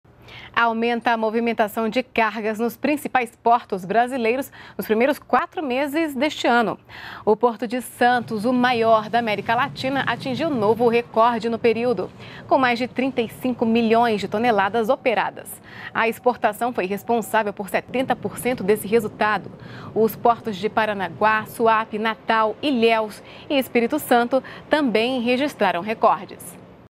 Cresce a movimentação de cargas nos principais portos brasileiros
NBR NOTÍCIAS - 22.05.15: Aumenta a movimentação de cargas nos principais portos do Brasil entre janeiro e abril deste ano. O Porto de Santos registrou recorde de 35,82 milhões de toneladas operadas - exportação foi responsável por 70% do resultado.